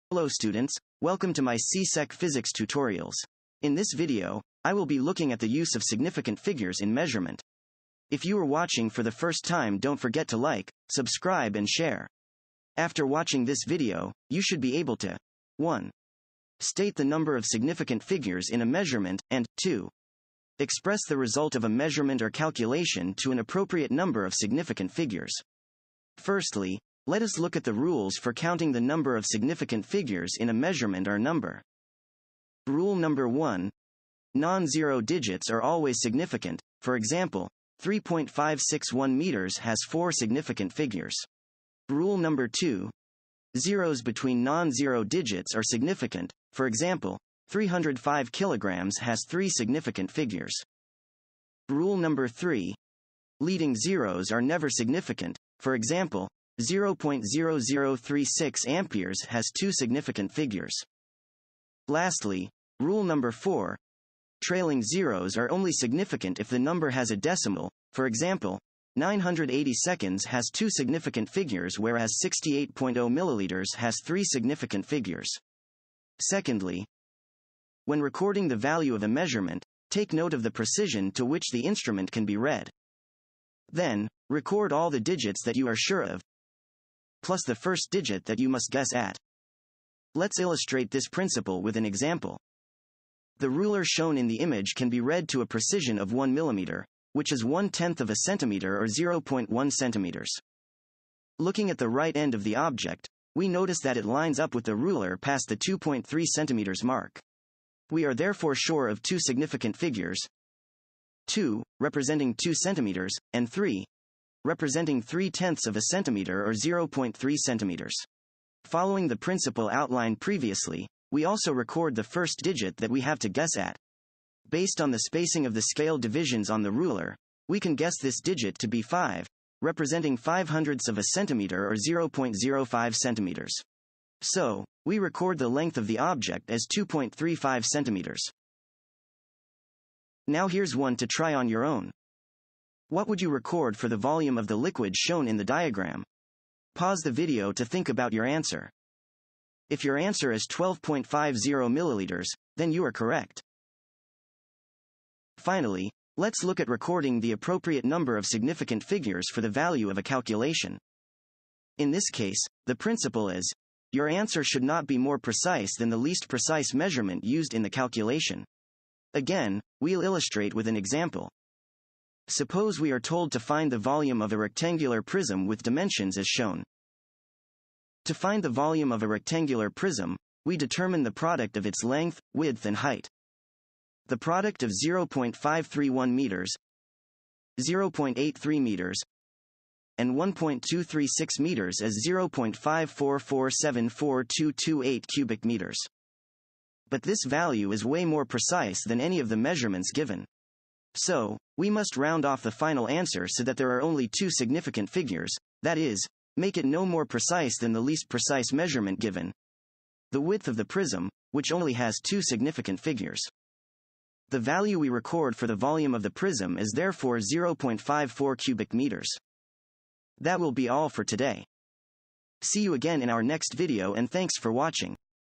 CSEC Physics Tutorial Significant figures
In this video, the use of significant figures in measurements and calculations is explored.

for tutoring in math, biology, chemistry and physics at the CSEC level.